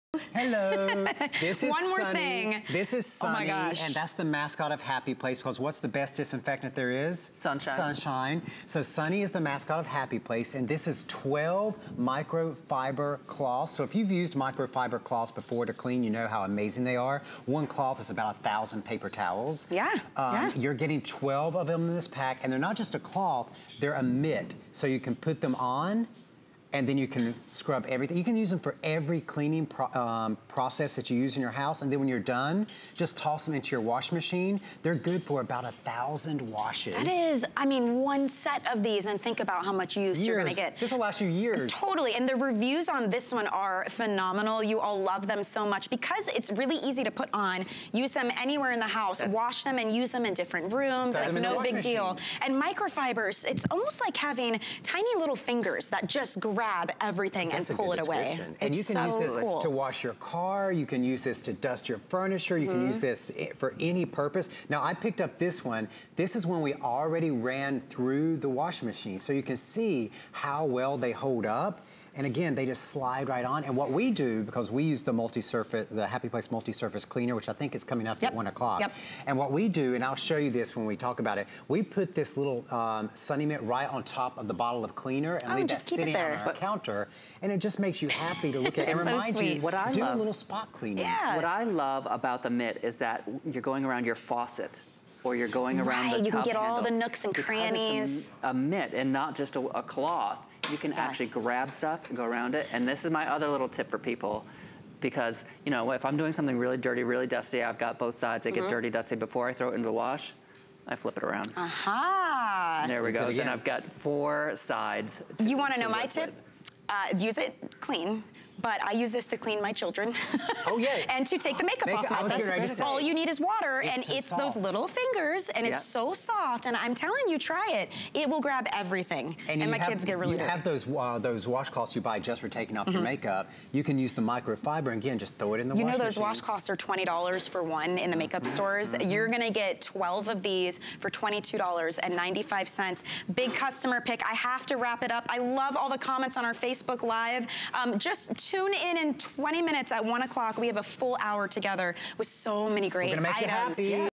Happy Place "Sunny" 12pack 8" Microfiber Cleaning Mitts
It's only happy cleaning with these reusable microfiber cloths. Featuring a mitt/pocket design, they can be used and stored over your cleaning product bottles and are machine washable. Use them with liquids or dry for wiping up messes and keeping your home spotless.

    12 Sunny Microfiber Cleaning Mitts